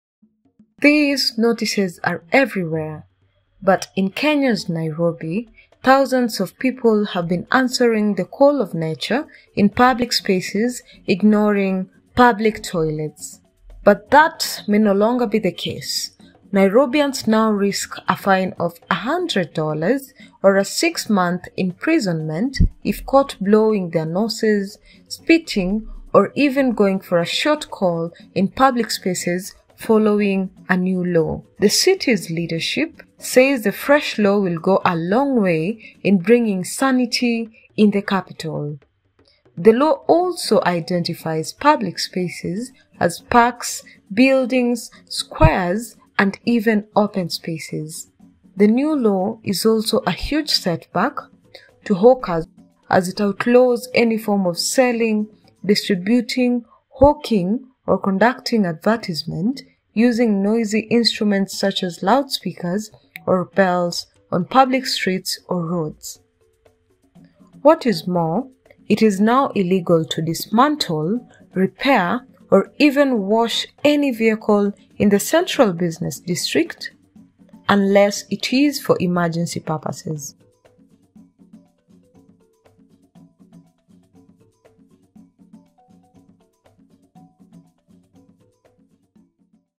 PEEING, SPITTING IN TO COST RESIDENTS $100
By Faith Nyasuguta

In Nairobi, urinating in an undesignated place will cost one a fine of $100 or six months of imprisonment following a new law.

The same fine is applicable when one blows their nose without a handkerchief or tissue paper.

Hawkers are also not allowed to sell their merchandise while shouting or using loud instruments.

Washing or repair of cars is also not allowed in the Central Business District unless it is an emergency.

"Nairobi is the face of Kenya. We all need a clean and hygienic environment to live and work in," Nairobi Governor Ann Kananu said.